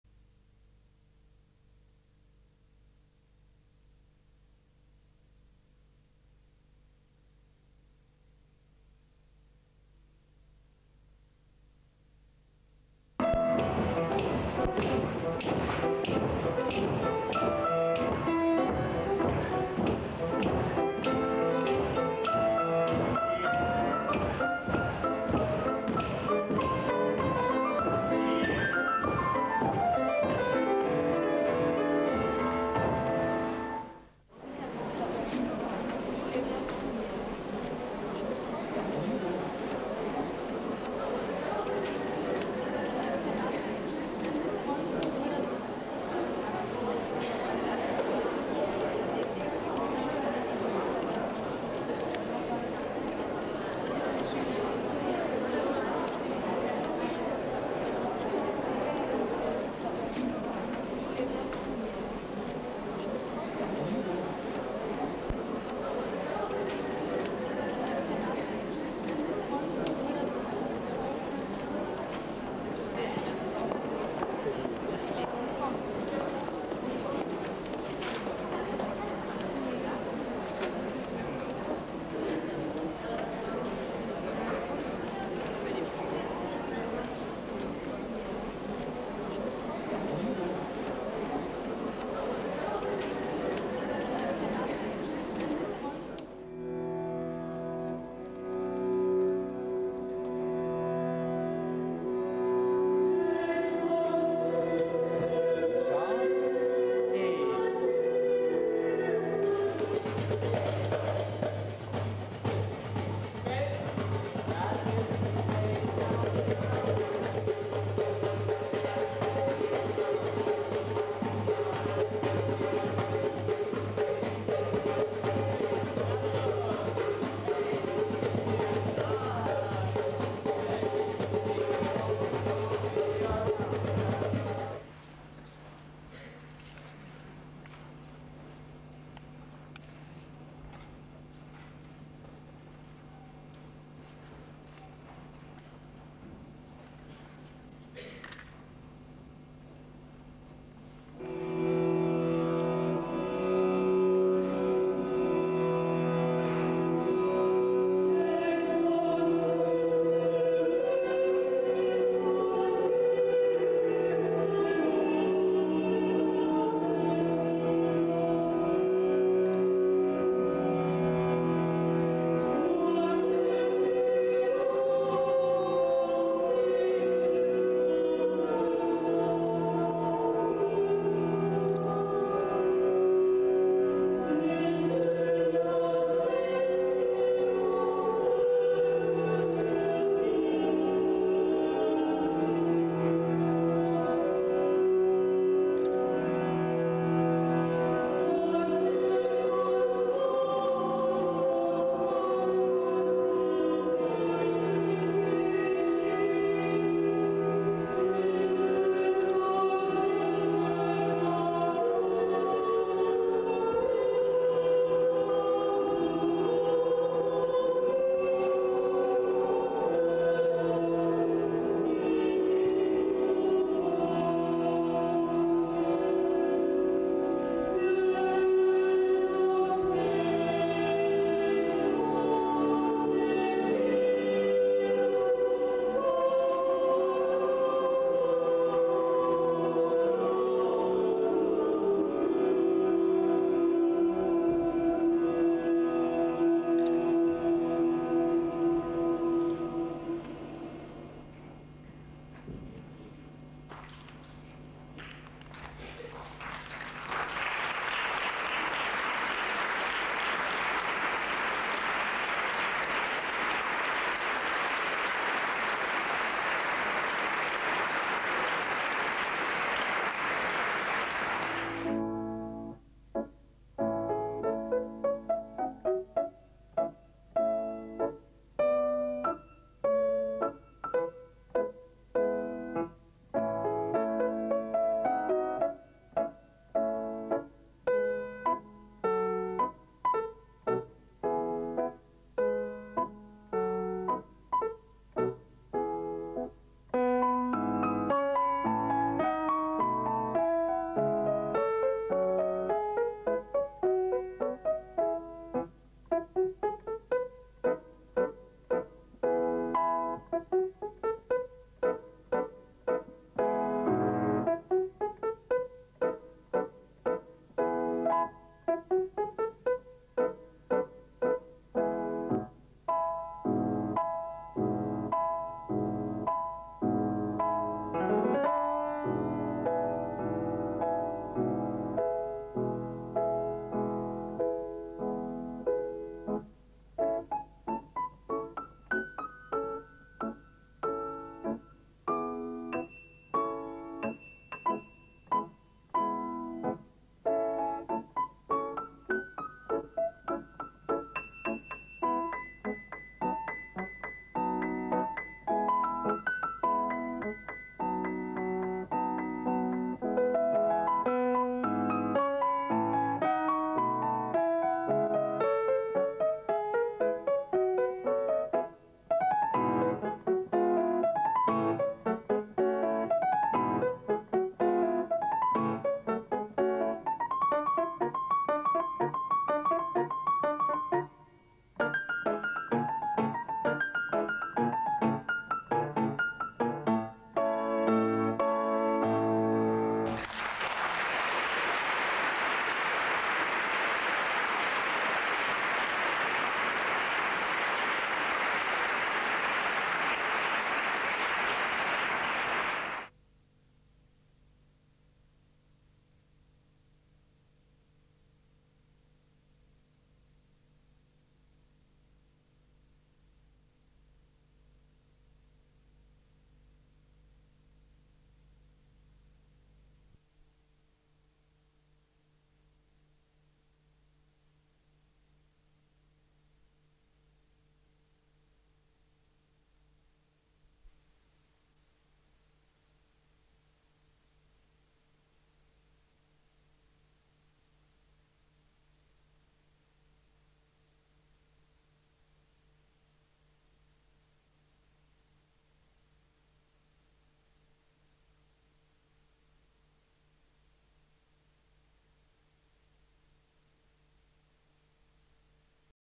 PRIX DE LAUSANNE 1999 - Nathalie Sonnenschine
Nathalie Sonnenschine (born as Natalia Maria Wojciechowska) one out of two female dancers (Katarzyna Gdaniec was first) was a finalist of Prix de Lausanne.
After her performance at this competition Natalia got scholarship to John Neumeier's ballet school.